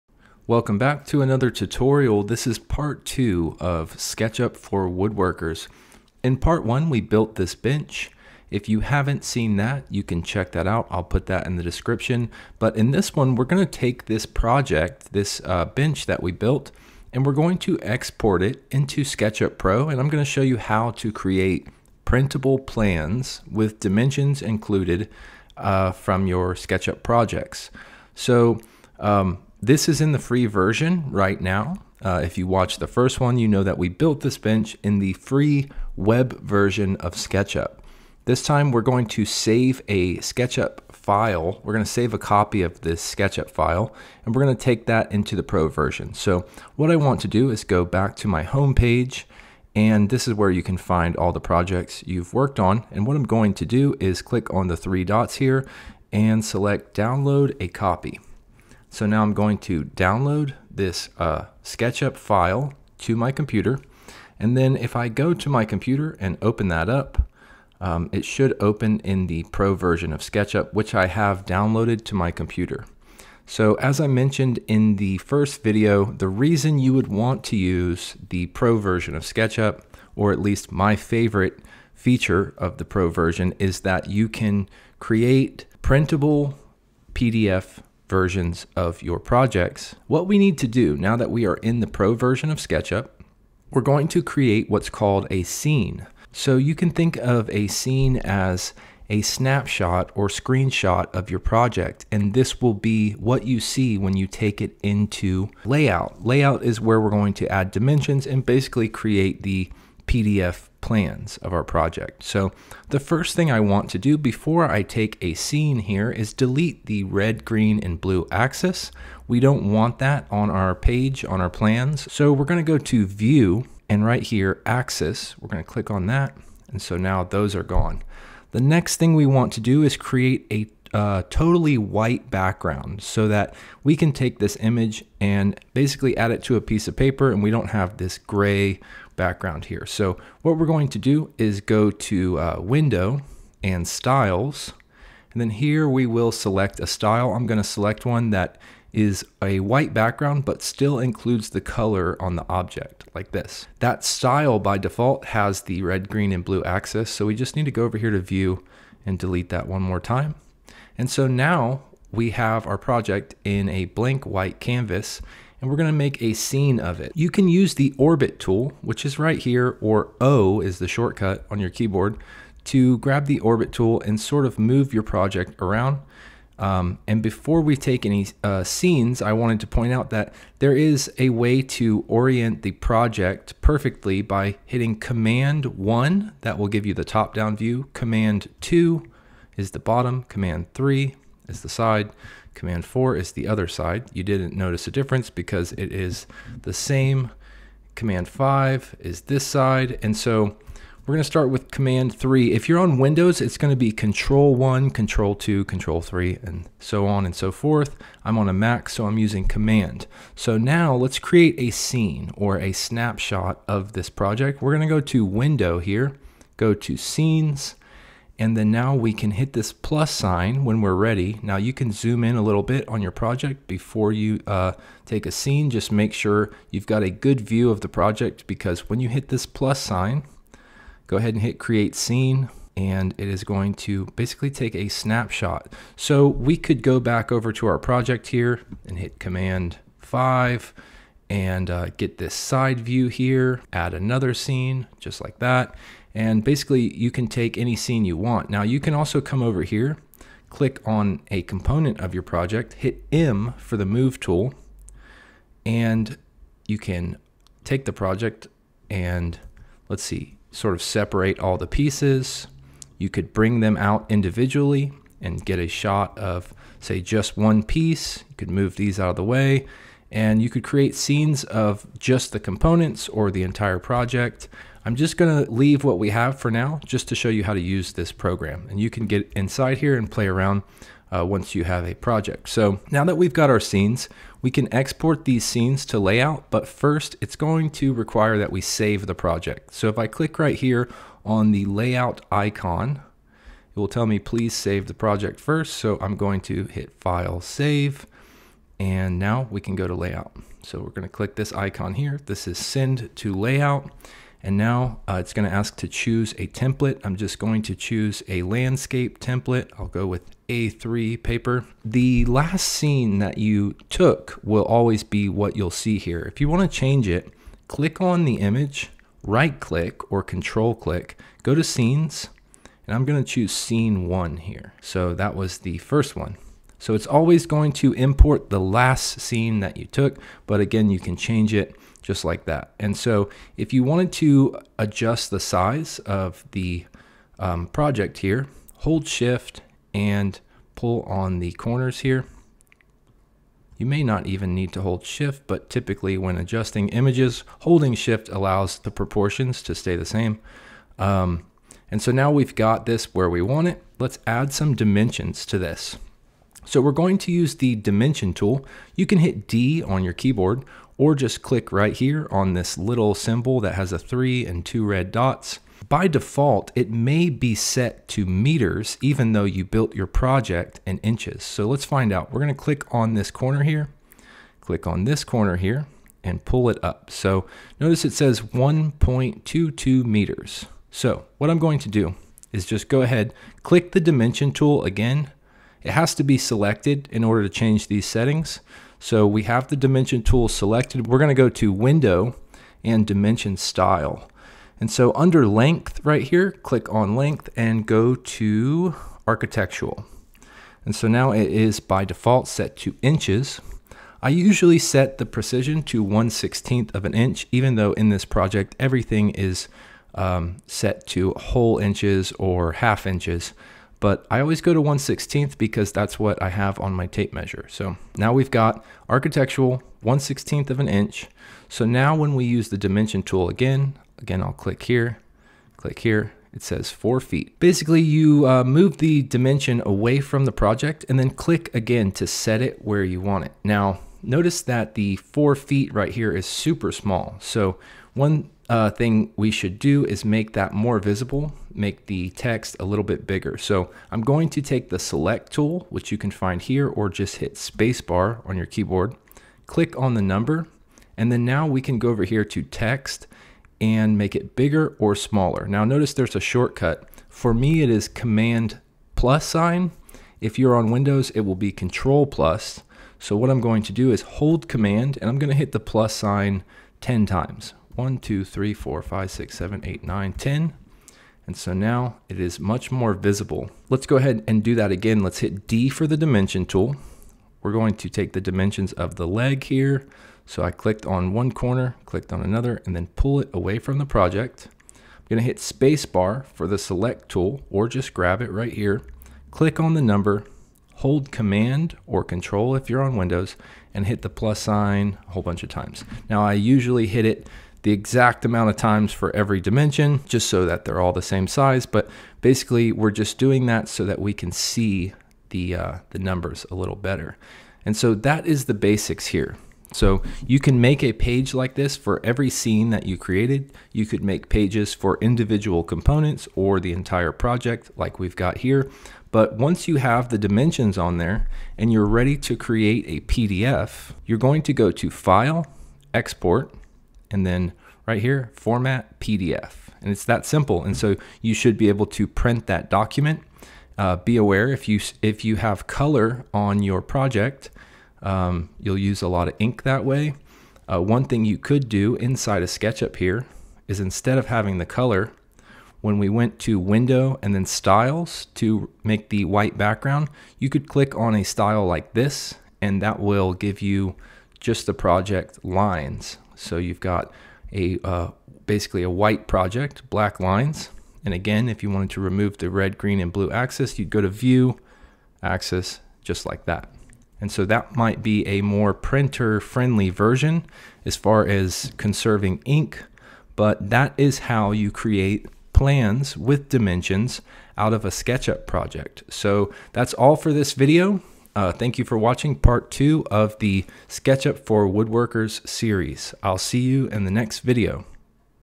Sketchup for Woodworkers - Part 2 - Printable Plans
Learn how to turn your woodworking projects into printable plans using Sketchup.

This is part 2 of Sketchup for Woodworkers. Below is a link to part 1.